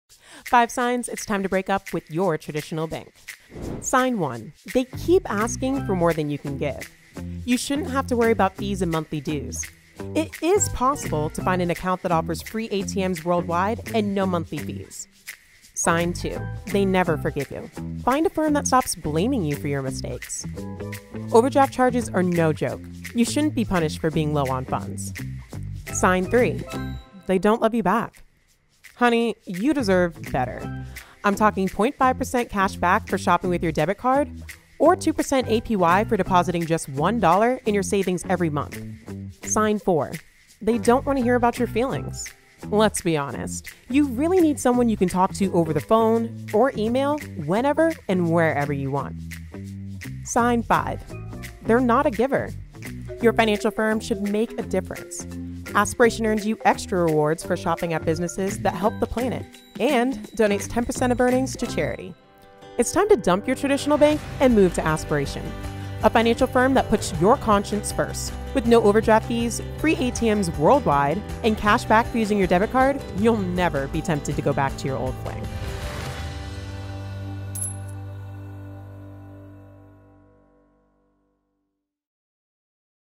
5 Signs It’s Time to Break up With Your Bank
Your bank giving you all kinds of bad feelings? It might be time to cut the toxicity out of your life. Here are five signs it’s time to break up with your bank.